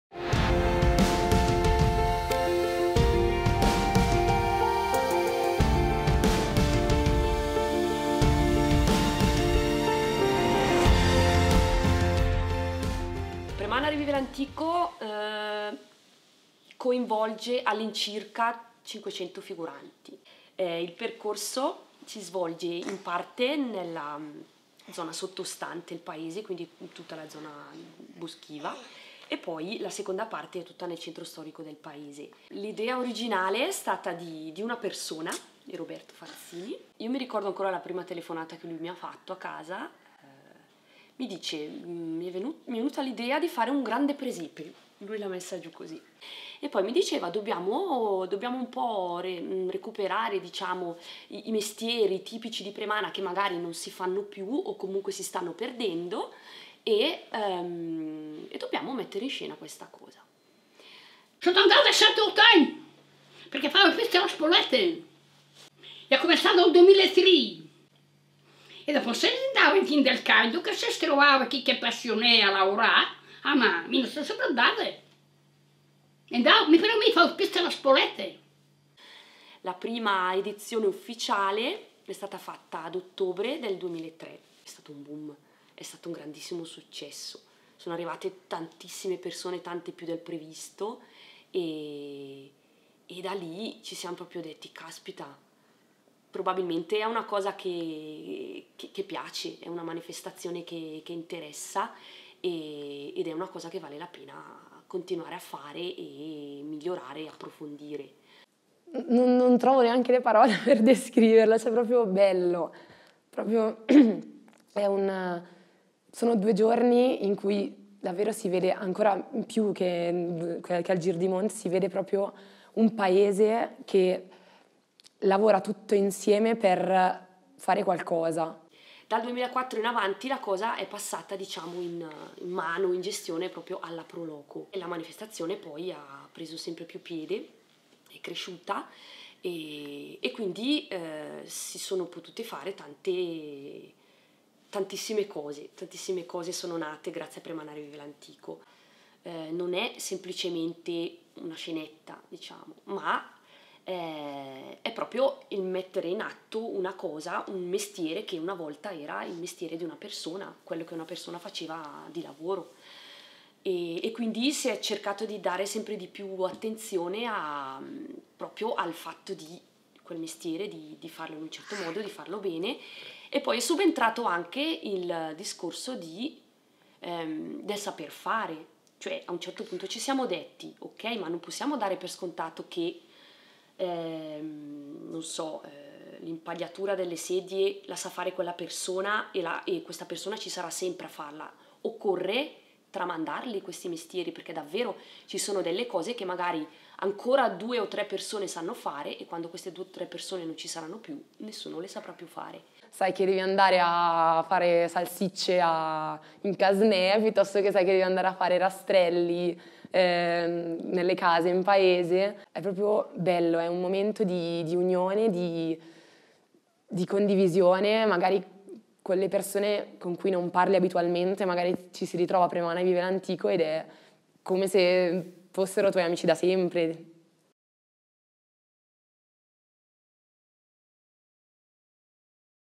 Premana Rivive l'Antico / Mappa di comunità di Premana
Un'importante manifestazione di rievocazione storica si tiene ogni due anni a Premana: Premana Rivive l'Antico.